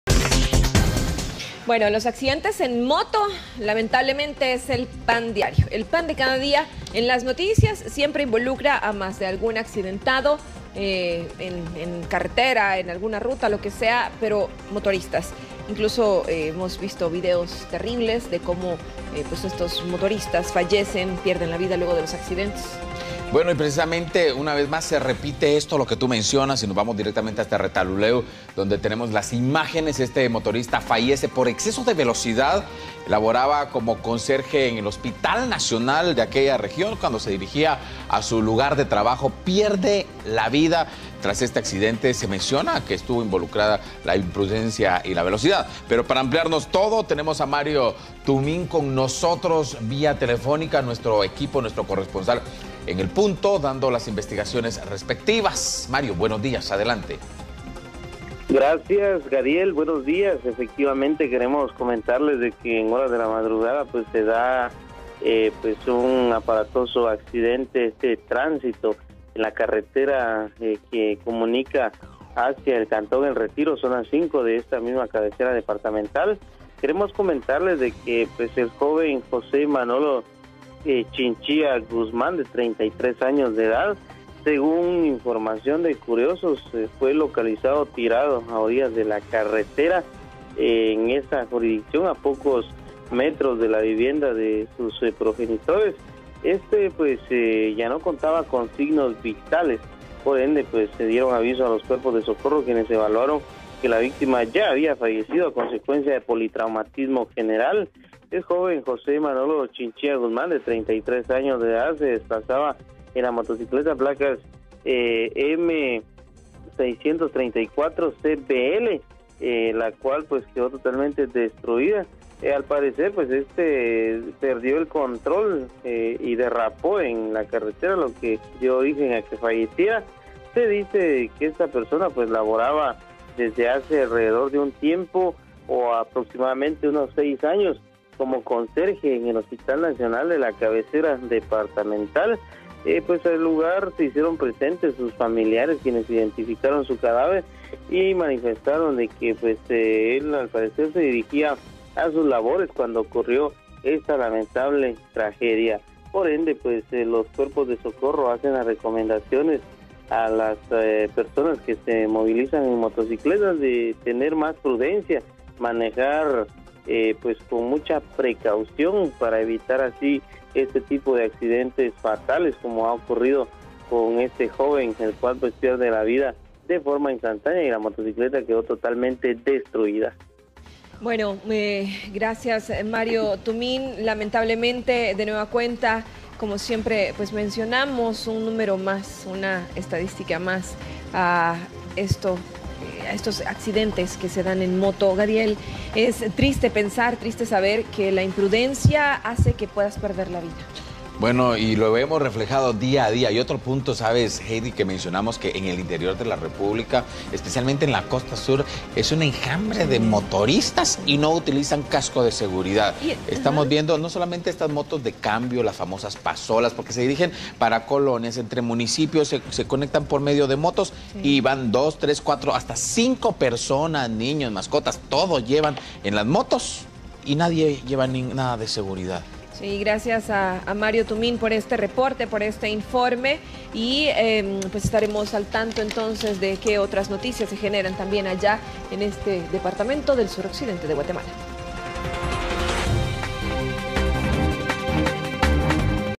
Motorista perdió la vida en un accidente vial
Murió por exceso de velocidad
La víctima de 33 años de edad laboraba como conserje en el Hospital Nacional de Retalhuleu y tuvo el fatal percance cuando se conducía rumbo a su lugar de trabajo.